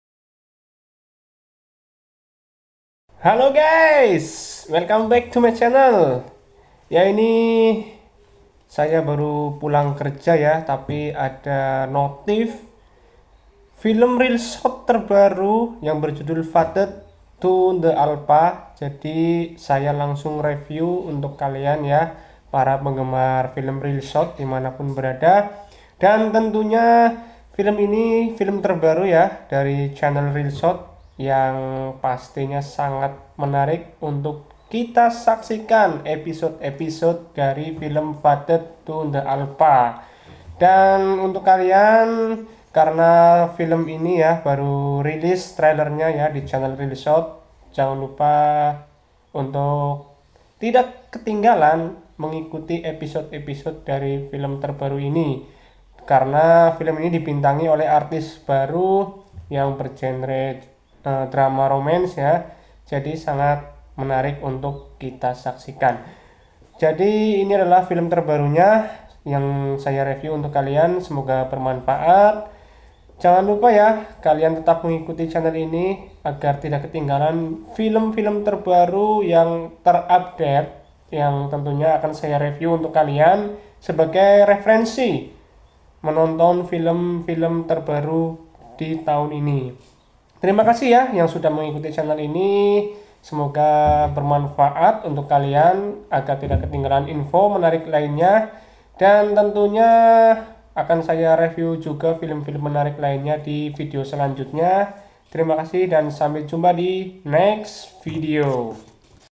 Janhan Lupa Saksikan Fated to the Alpha ya guys
Review film reelshort romance terbaru yang berjudul Fated to The Alpha. jangan lupa saksikan episode terbarunya ya